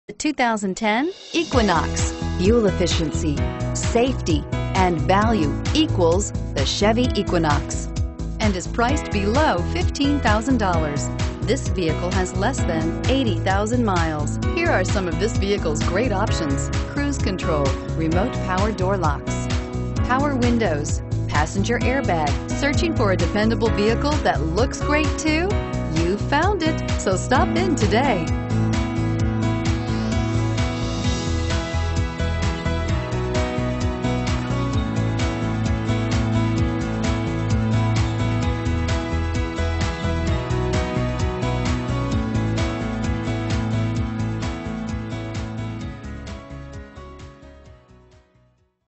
2010 Chevrolet Equinox Midland Ontario 14590A
2010 Chevrolet Equinox LT

Tom Smith Chevrolet
824 King St.
Midland Ontario L4R 0B8

Don't miss this 2010 Chevrolet Equinox. It's equipped with Automatic transmission and features a Black exterior. With 75478 miles, You'll want to take this car home. Make a great choice today.